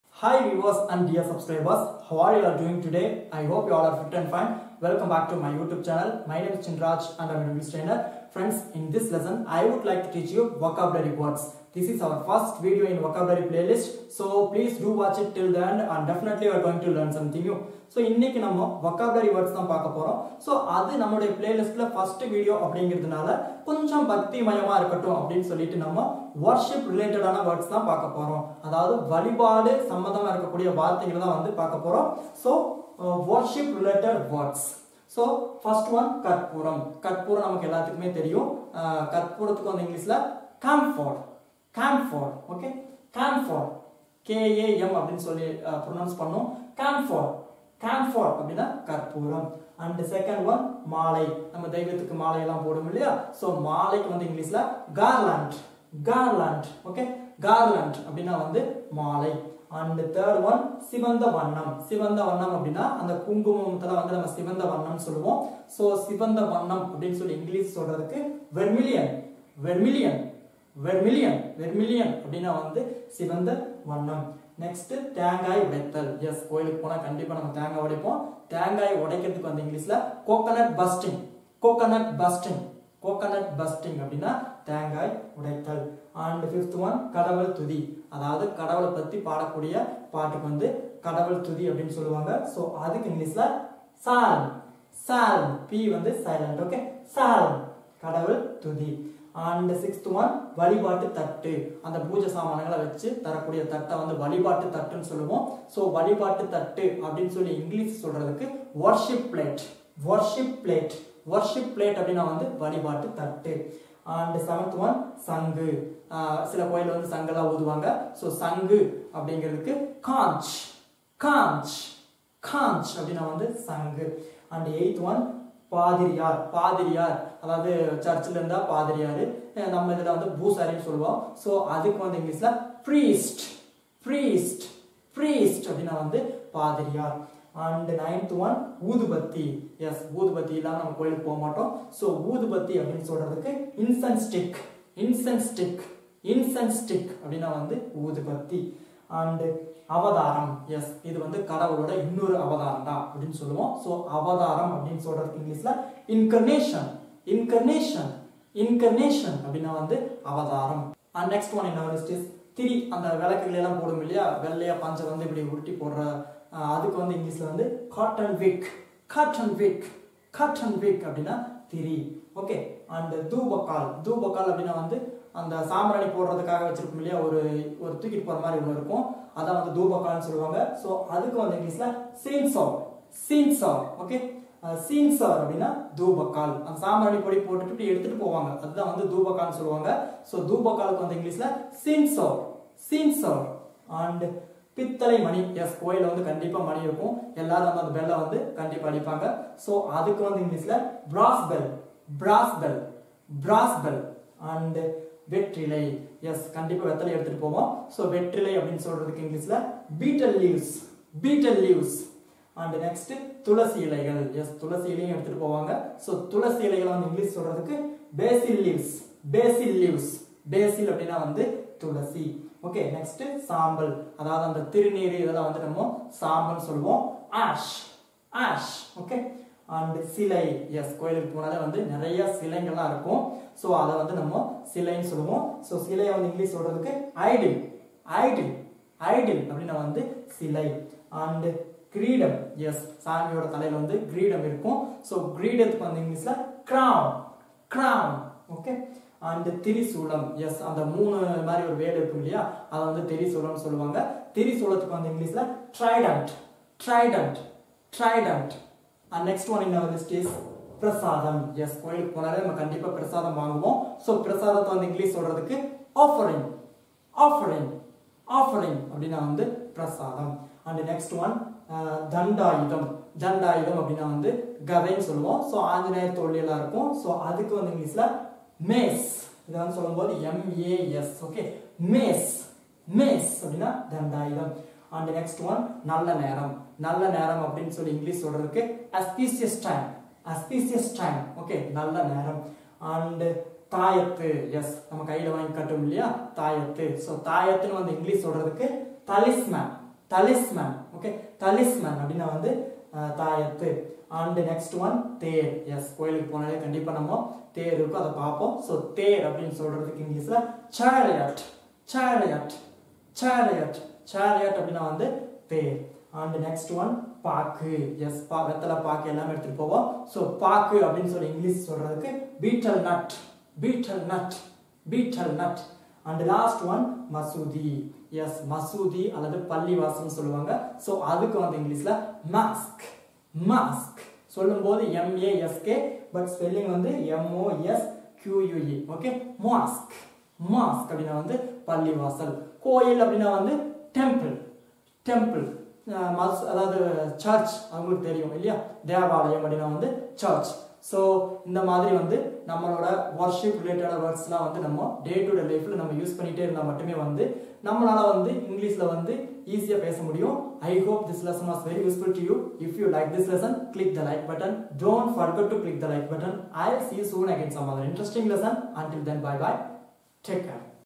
worship related vocabulary words in English through Tamil
Hi friends welcome to our spoken English channel this video we are going to learn  some worship related vocabulary words in English through Tamil